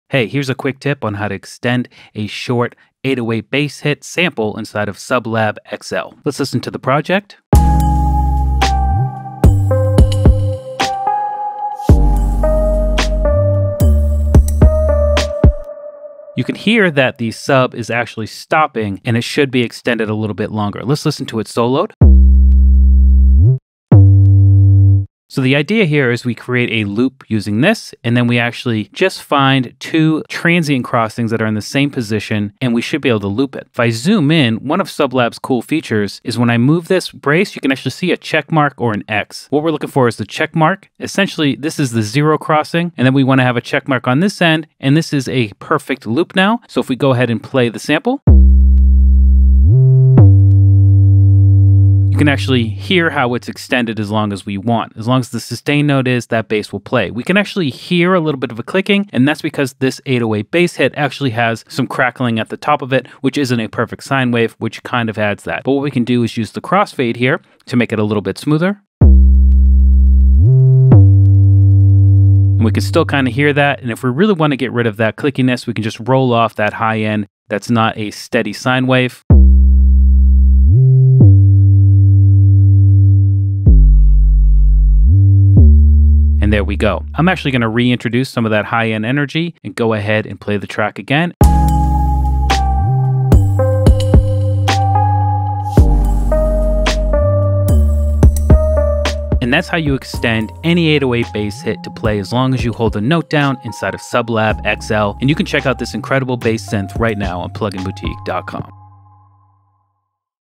SubLab XL - Working with 808 Samples - Endless Sustain on Short Clips - Quick Tip
In this quick tip video tutorial, Joshua Casper shows you how to use a short 808 bass sample in SubLab XL by Future Audio Workshop and make it have an endless sustain. You do this in SubLab XL's sample module using the Loop function, X-fade and filtering if need be. SubLab XL's loop function has a super cool built-in function that will show you exactly where the loop in and out points need to be for a perfect seamless loop!